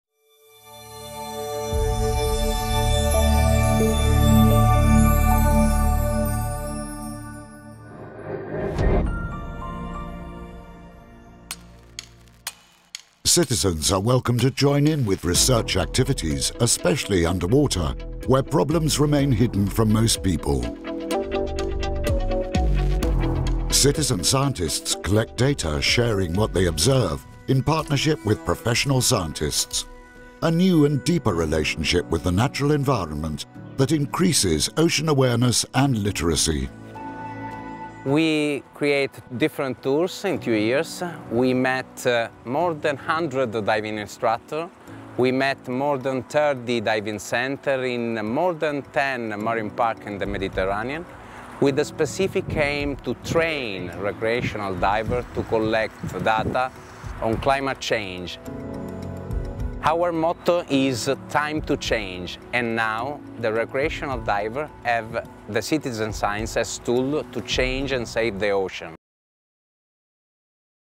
2. CITIZEN
Climate-related impacts under the sea remain hidden from most people. CitizenScientists workingtogether can help climate research on Mediterranean marine ecosystems.

Join our research activities underwater!

This pill is part of the series "MED Together" produced by the Interreg Med MPA Engage and MPA NETWORKS projects.